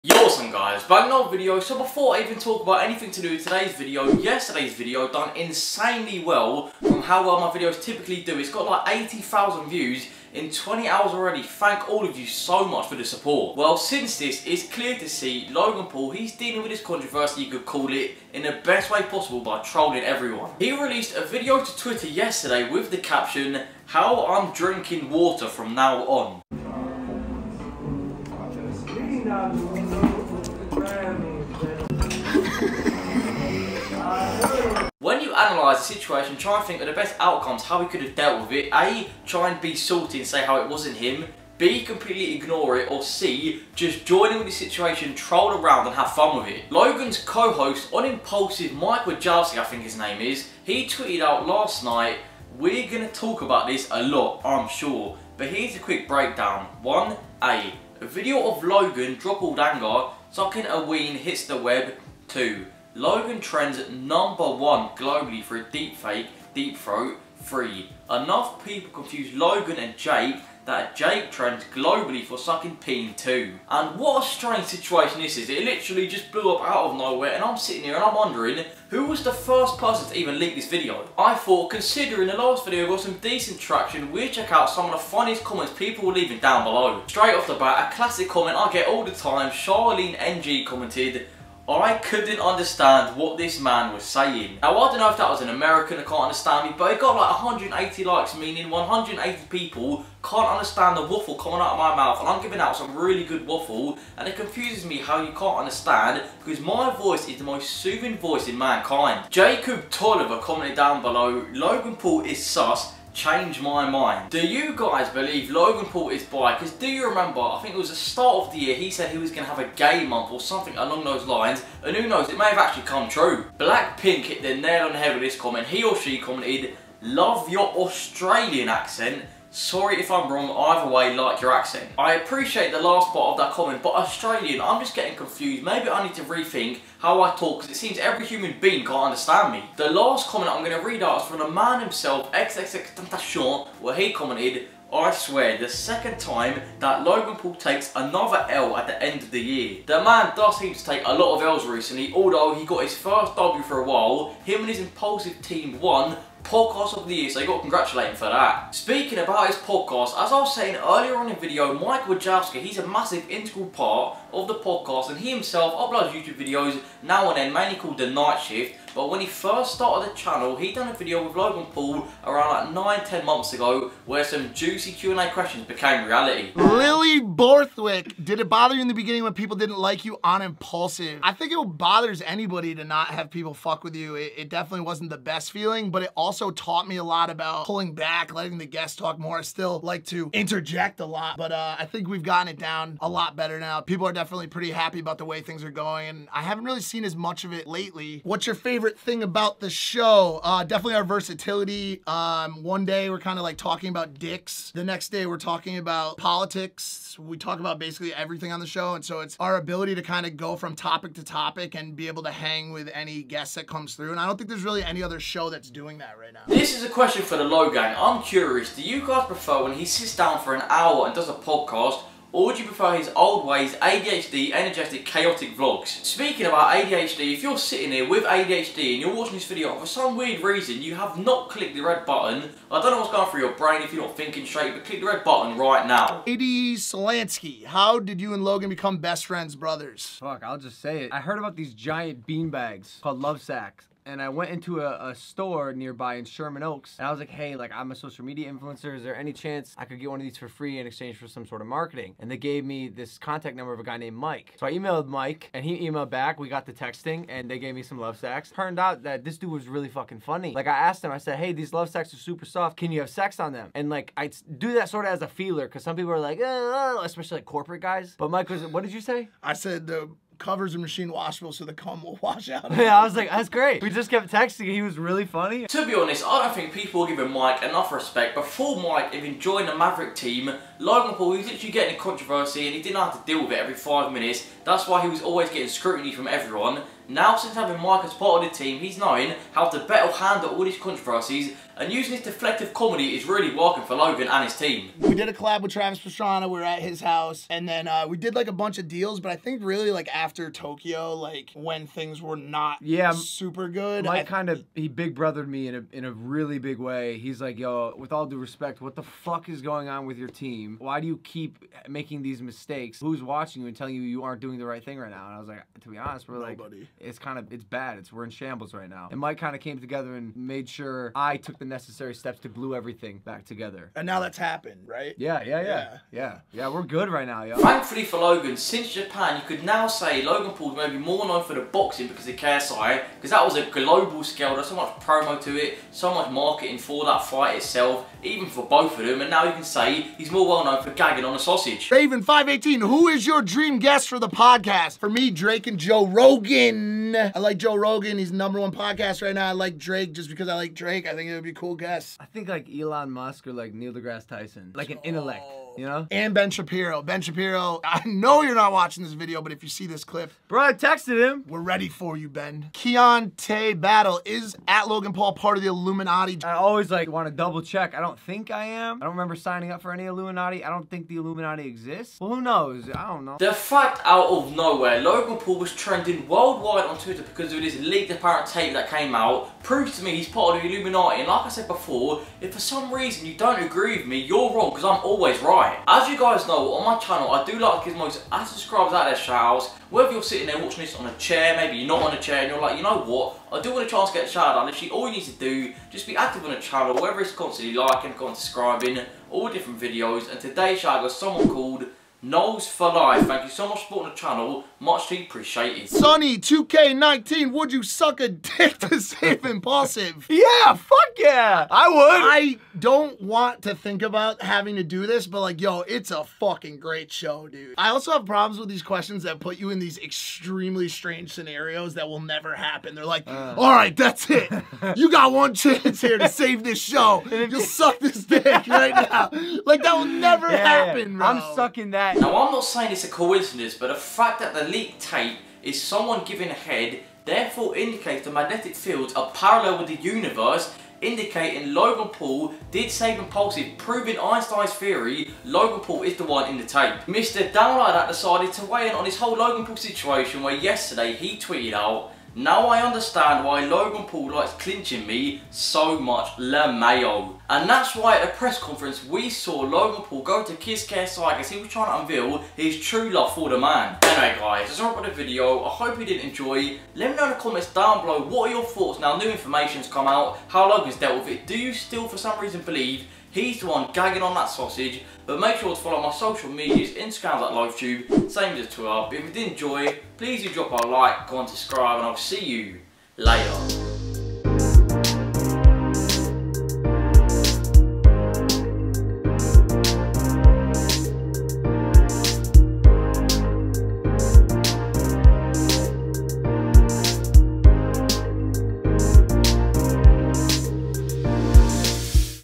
Logan Paul Admits S3X Tape Is Real (Actual Footage)
In todays video I talk about the alleged leaked tape to do with Logan Paul gagging on sausage. Over the past few days on Twitter he's been trending due to rumours of an alleged tape going viral.